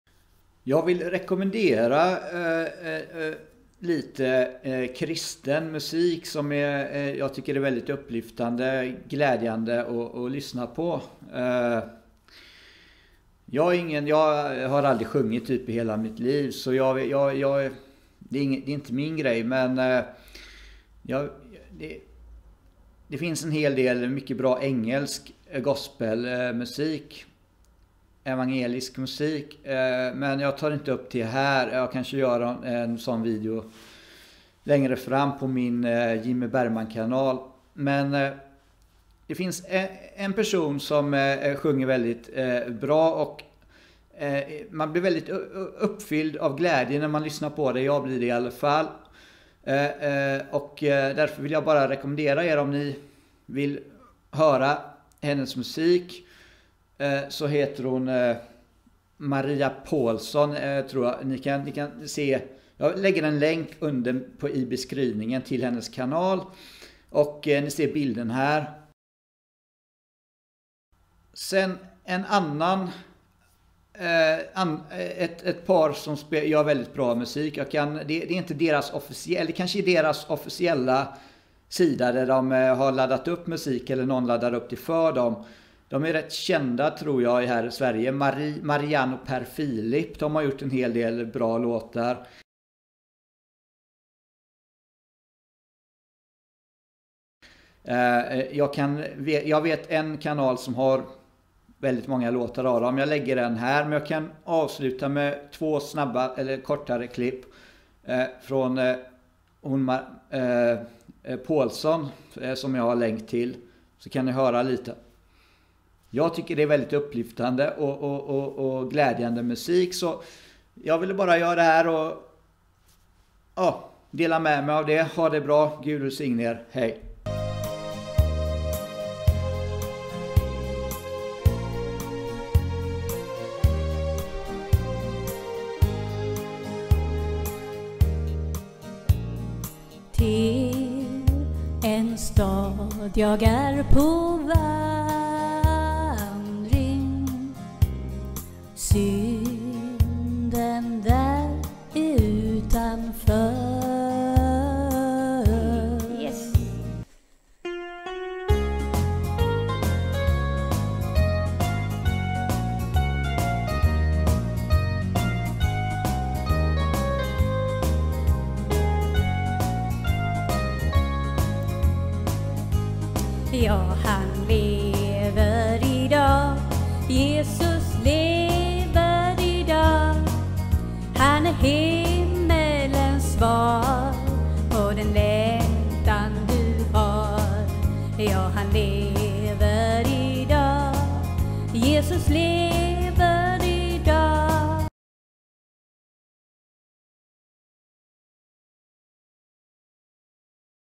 Bra svensk Gospel musik jag rekommenderar
Vet att vissa letar efter mer kristen musik, kanske gillar du dessa!